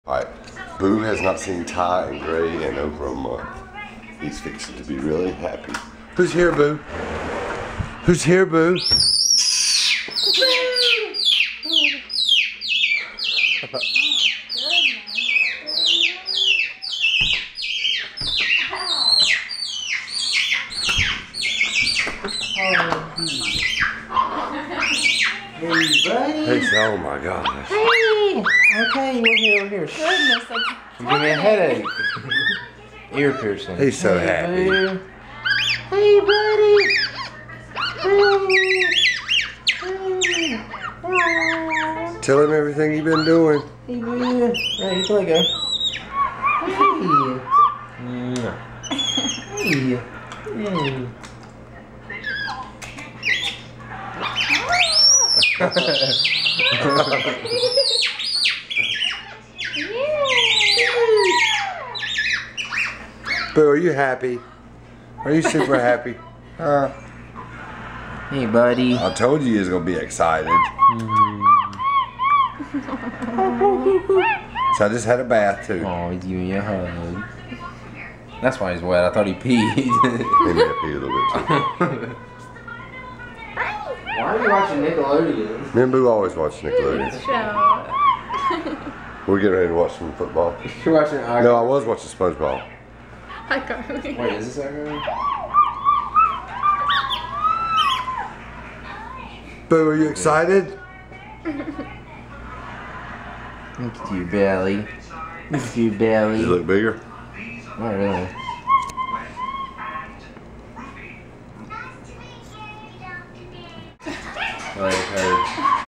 Monkey Freakin HAPPY!
MonkeyBoo is so happy to see Ty and gray after they been out of town for a month!